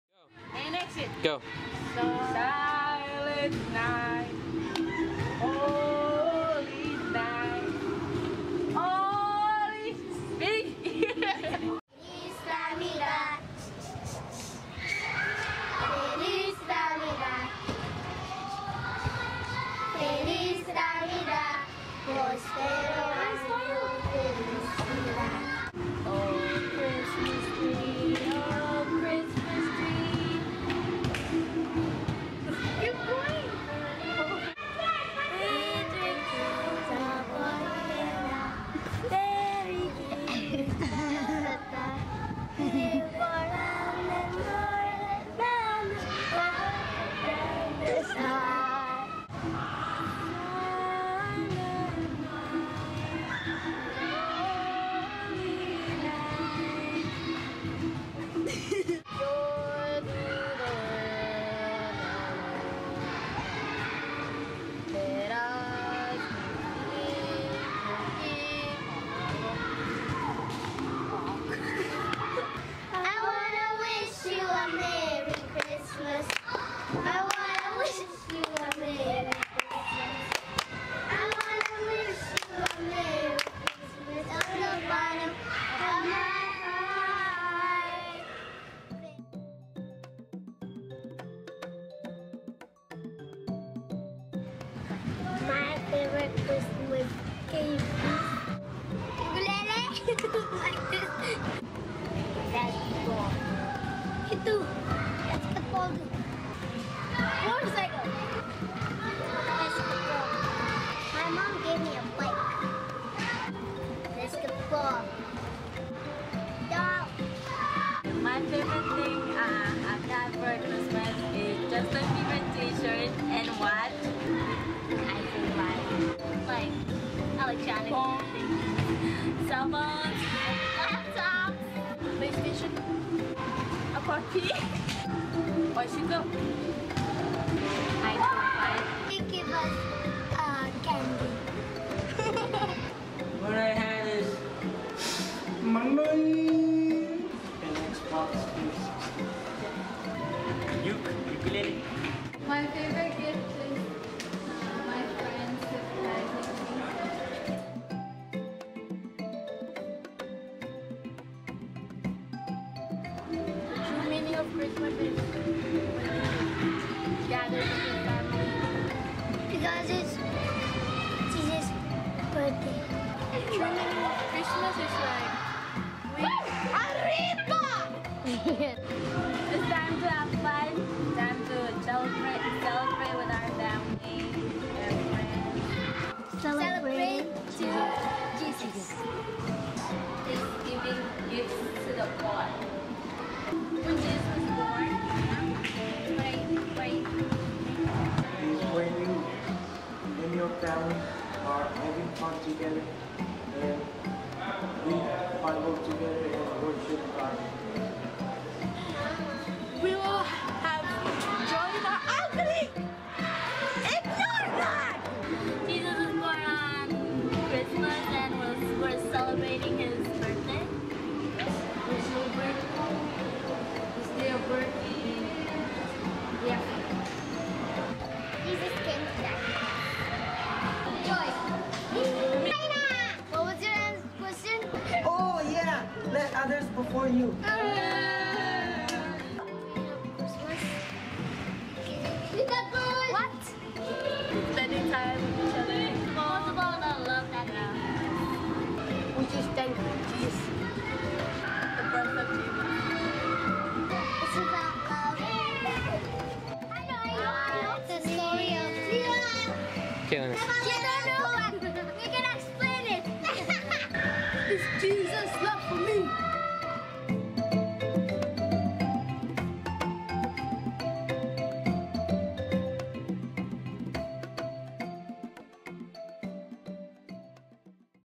The True Meaning of Christmas
Students at Delap SDA share what there favorite Christmas gift has been and what the true meaning of the holiday is all about.